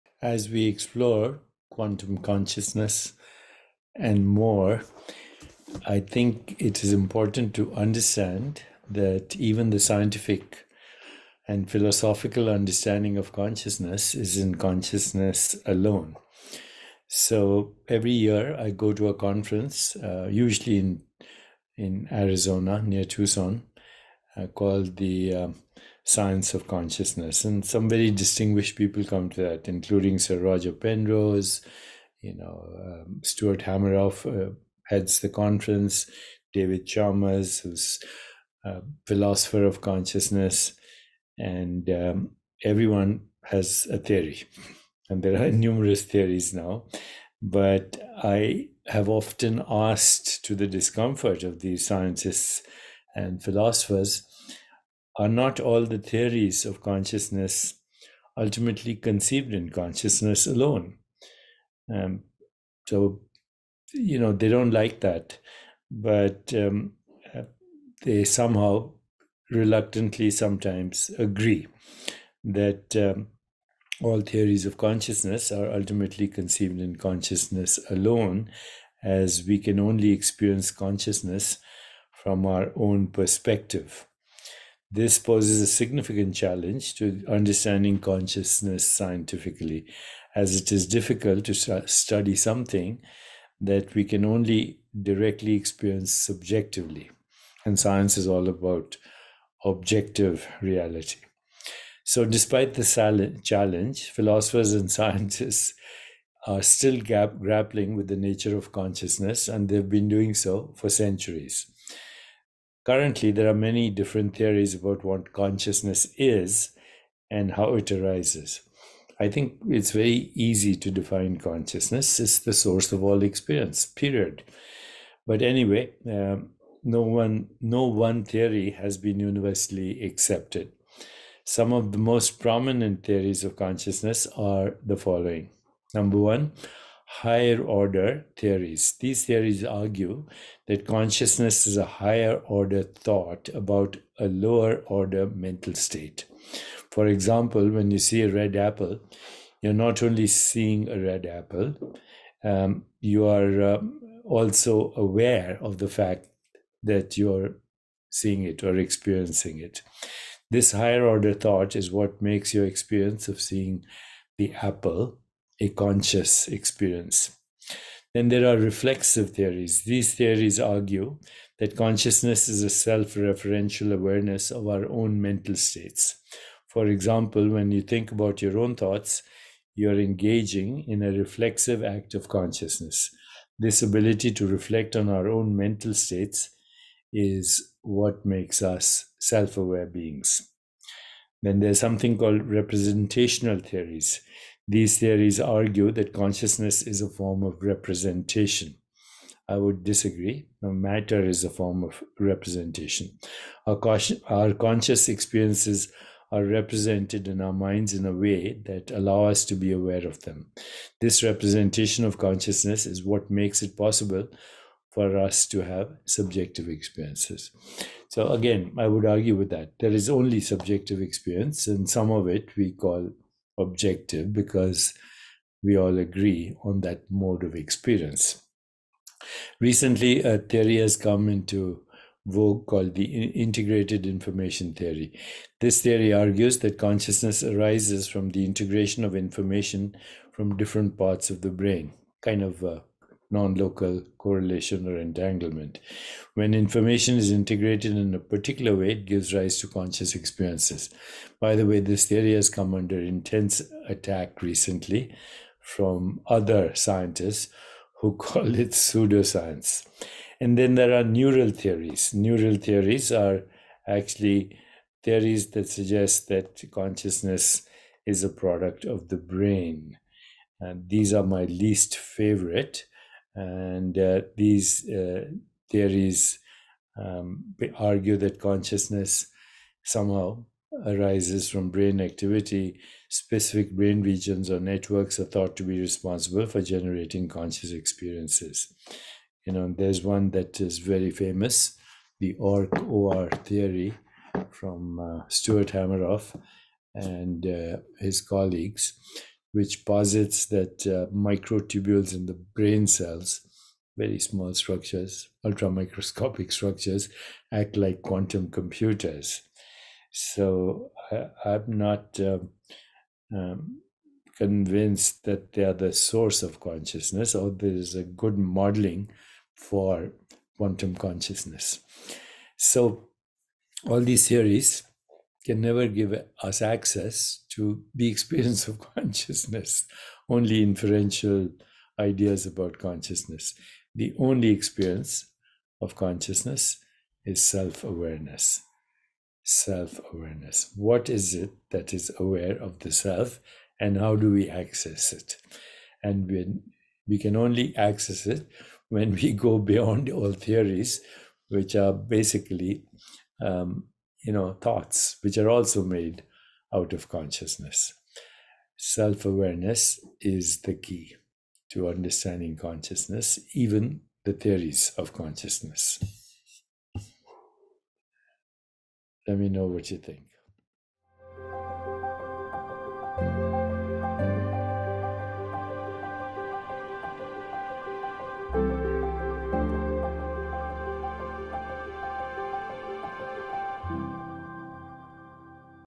The truth about scientific and philosophical theories of consciousness.
Scientific and Philosophical theories of Consciousness, including Quantum Consciousness, do not give us access to Consciousness.

Get a copy of Quantum Body: The New Science of Living a Longer, Healthier, More Vital Life at your favorite retailer